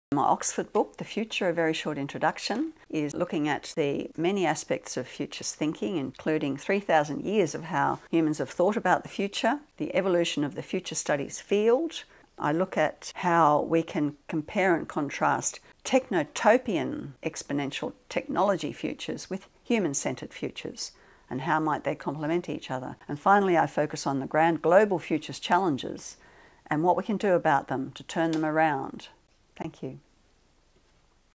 The Future: A Very Short Introduction
A very short video about my Oxford book: The Future: A Very Short Introduction. Now also available in Arabic, Turkish, Italian and Greek.
Licensed translations in Mandarin & Japanese are on the way.